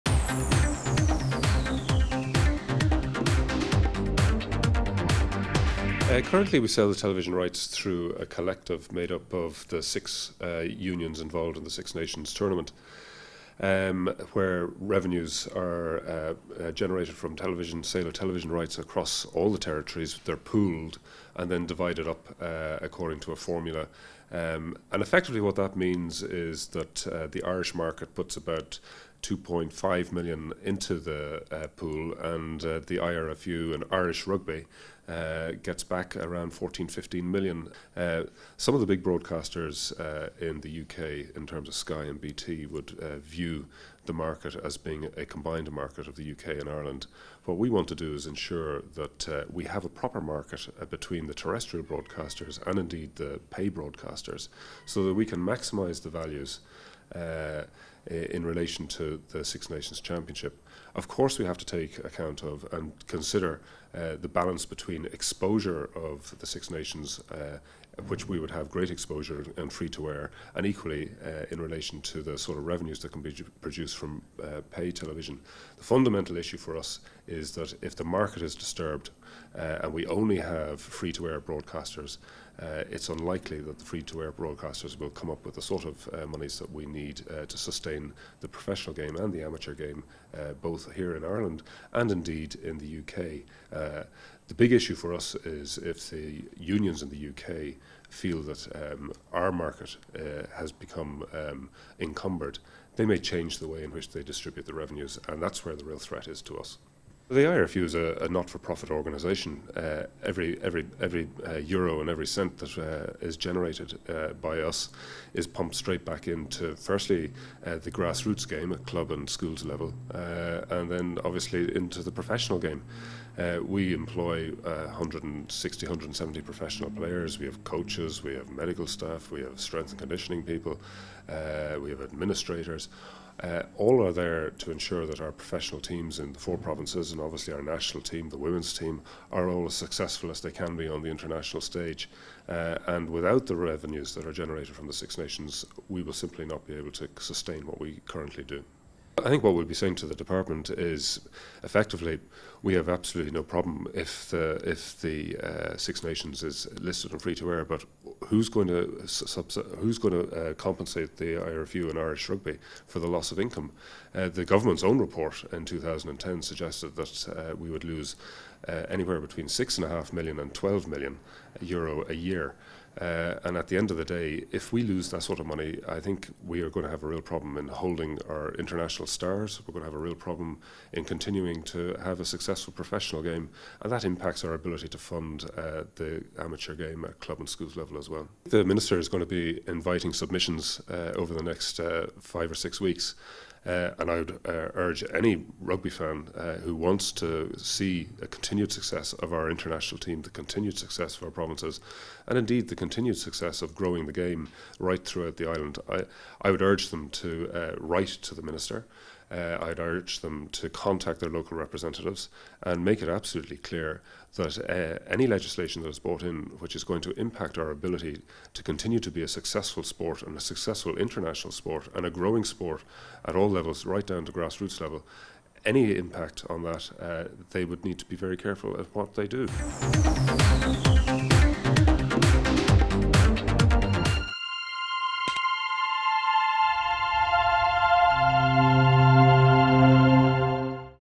Irish Rugby TV: Philip Browne On TV Rights
IRFU CEO Philip Browne speaks about the need for balance in the TV rights market and the importance of maintaining the current position to allow Irish RUgby to maintain their investment in the professional game at grassroots